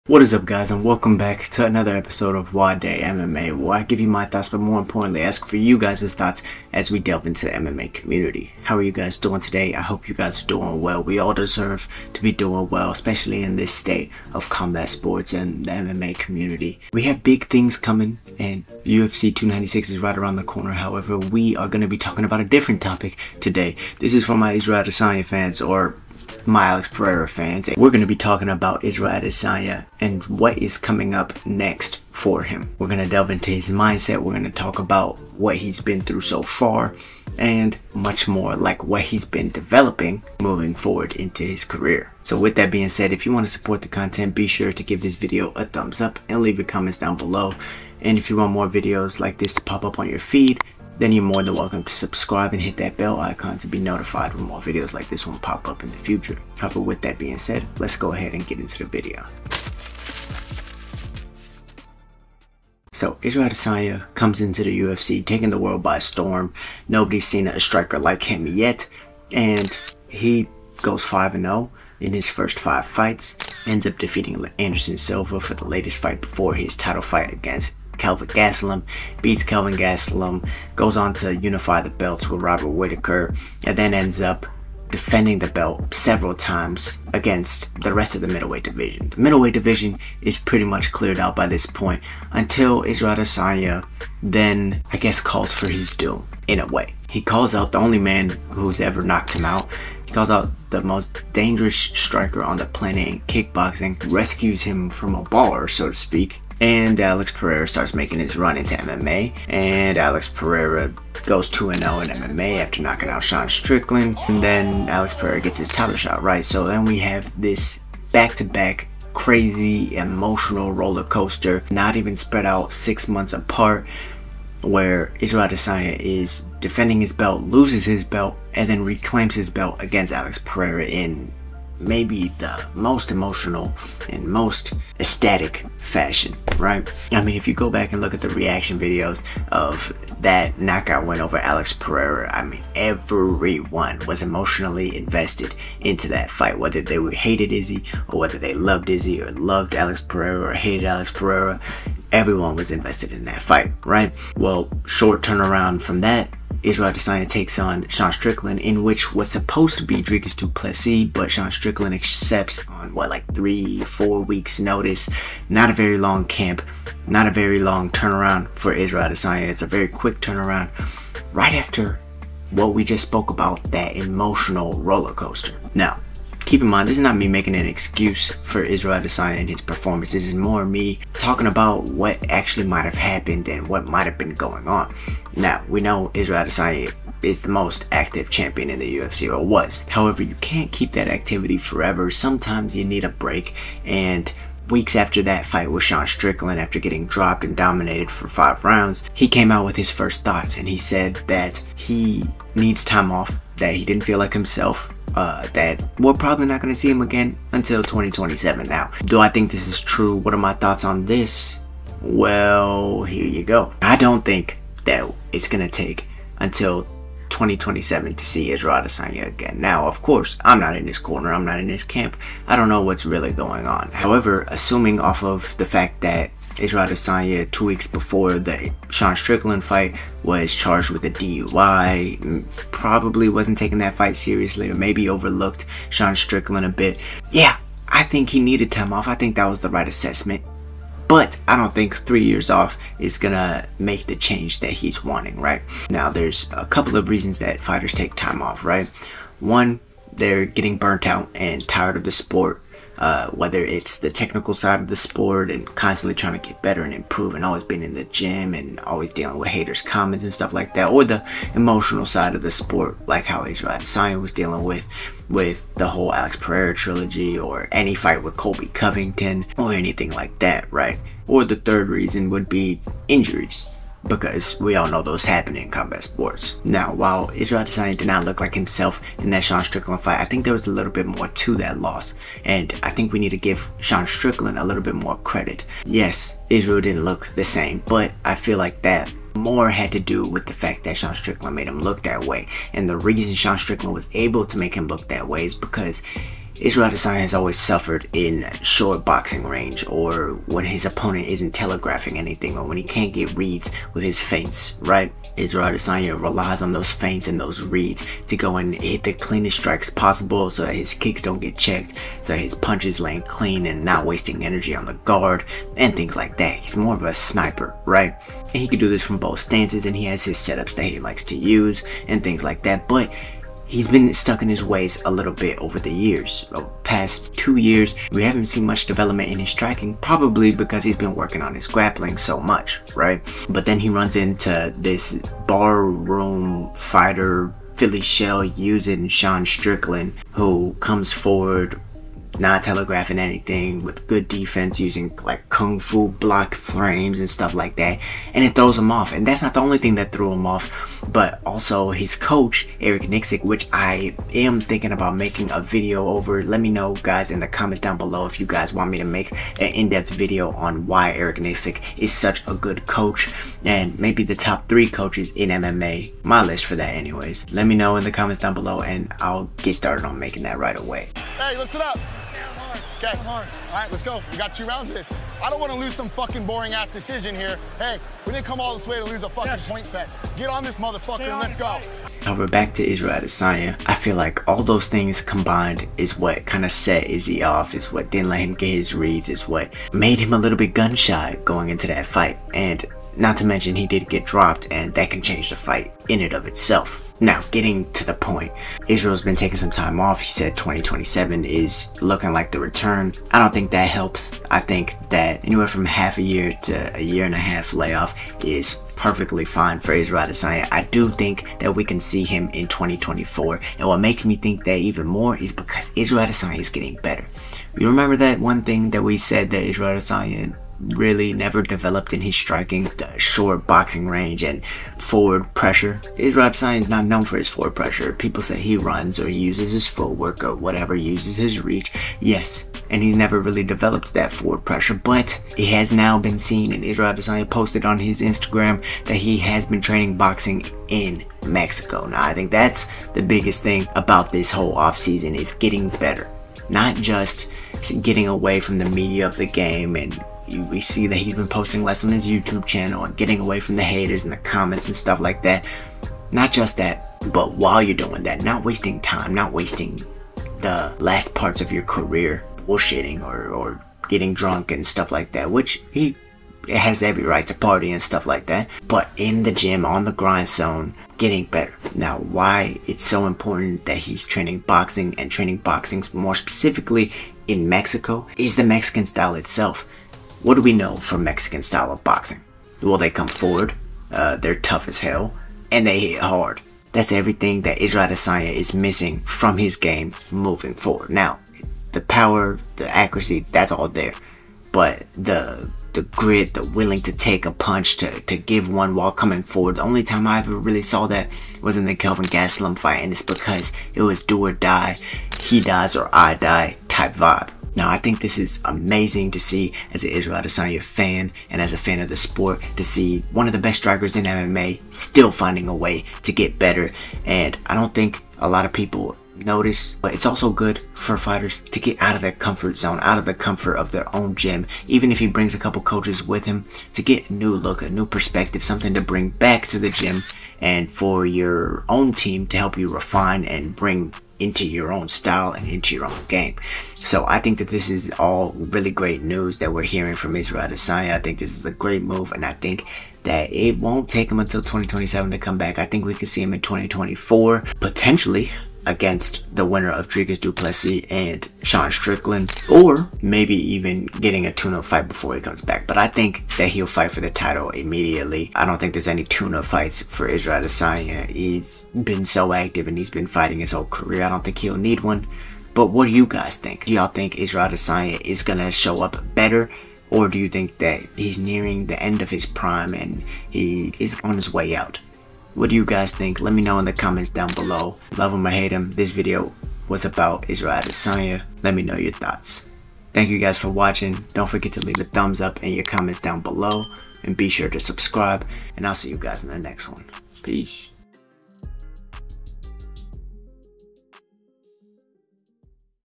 WHY IS ISRAEL ADESANYA TRAINING BOXING IN MEXICO??!! | The TRUTH Behind The Layoff!! | Nwadei MMA
In this video I will give you an update onto what Israel Adesanya may be working on, with his time off from the UFC, after his fight with Sean Strickland.

If you want a In-depth analysis about any of these fighters then PLEASE let me know! Welcome back to Nwadei MMA.

Don't forget to turn post notifications ON!